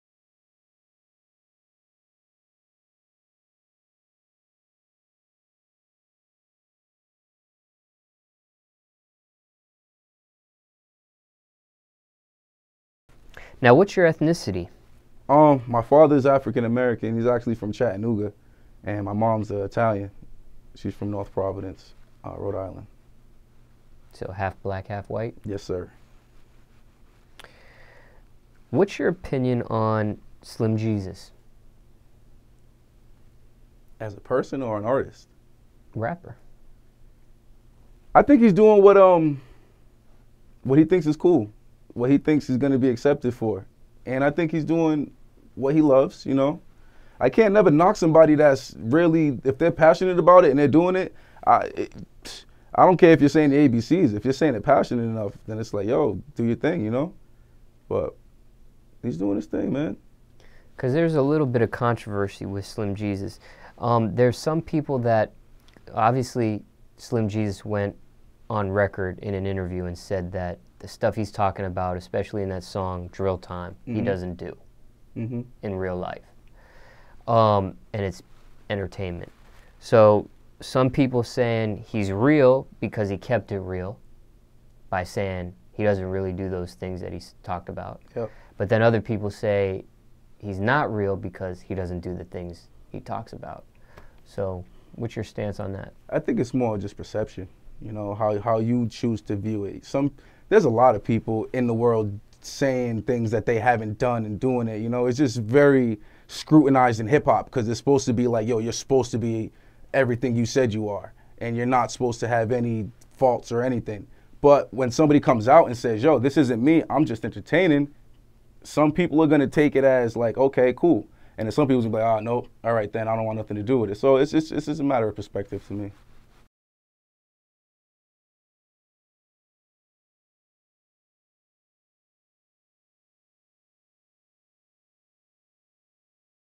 Bron Weighs In On Slim Jesus: It's A Matter Of Perspective To Me
Pawtucket, Rhode Island rapper and Yardmon/F.A.M member, Bron, sits down with DJ Smallz and weighs in on Slim Jesus' music, which has caused a stir in the hip hop community.

Do you agree with Bron on Slim Jesus' music?